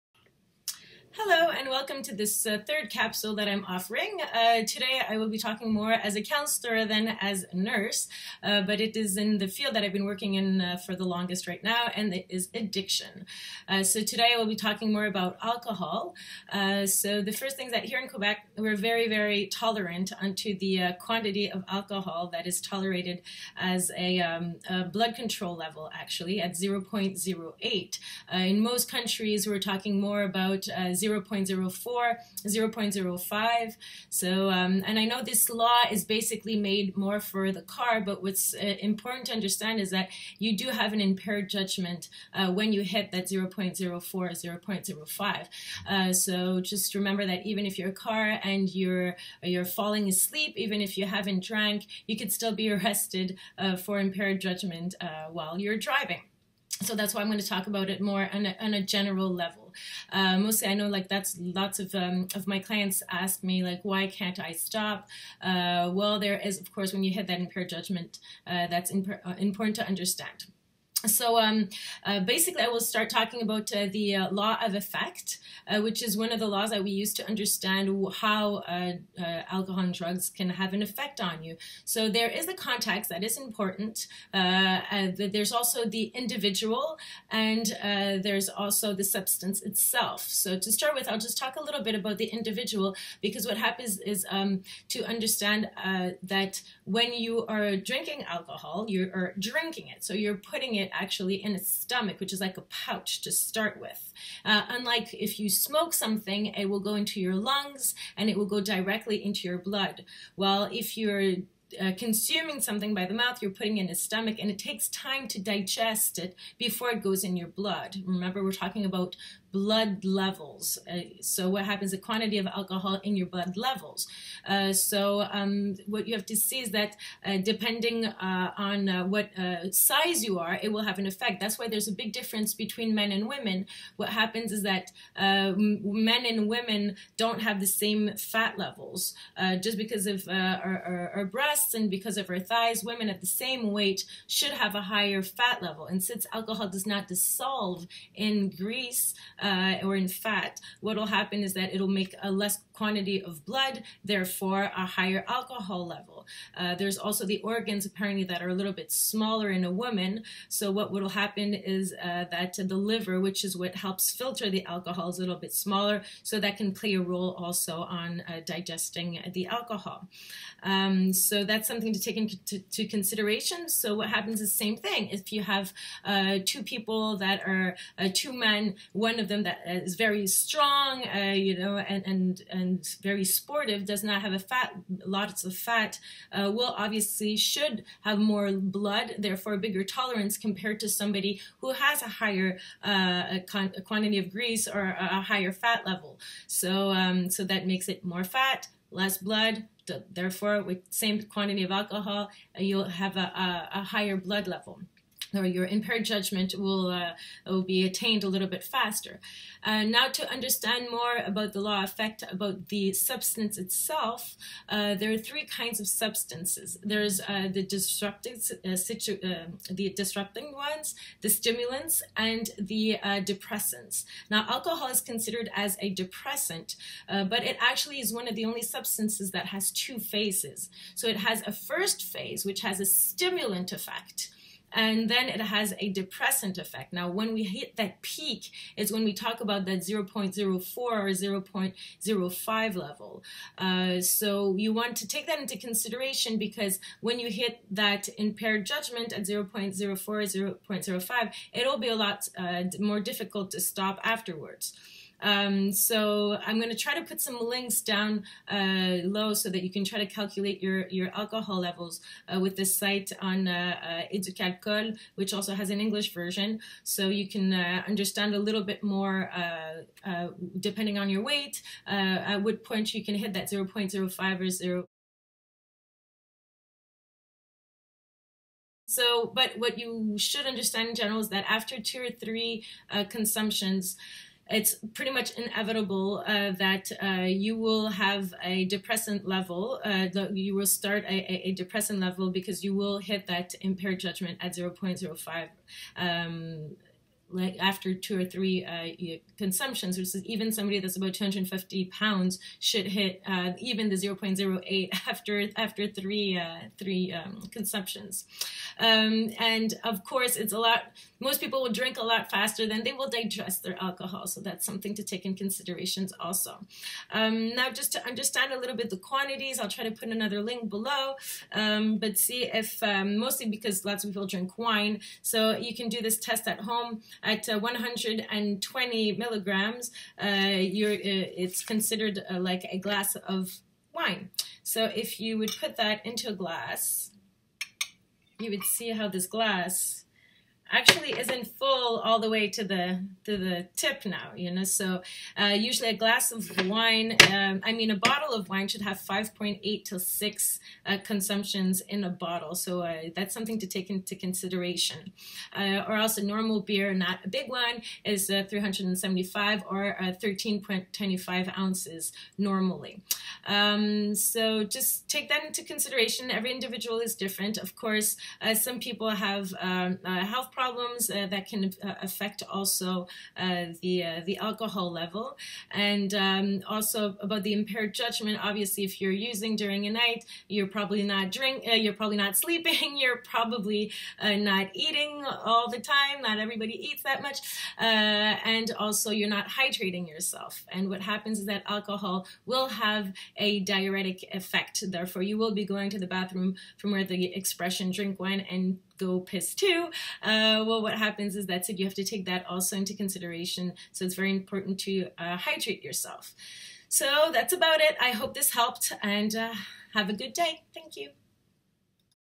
Ask the nurse - Blood-alcohol level En
Third informal video for people who would like to understand more on the effects of alcohol = blood-alcohol level. In Quebec we are very tolerant toward driving under the influence and this can create abuse. Le goal is to maintain a blood-alcohol level that is pleasing = moderation is always in good taste ;)

Éduc Alcool has a tool to calculate your blood alcohol level. In case of doubt, if you are tired, if you have physical or mental health issues ... respect your limits :

In case of an emergency that doesn'T need medical assistance in Quebec- Canada (located on the island of Montreal)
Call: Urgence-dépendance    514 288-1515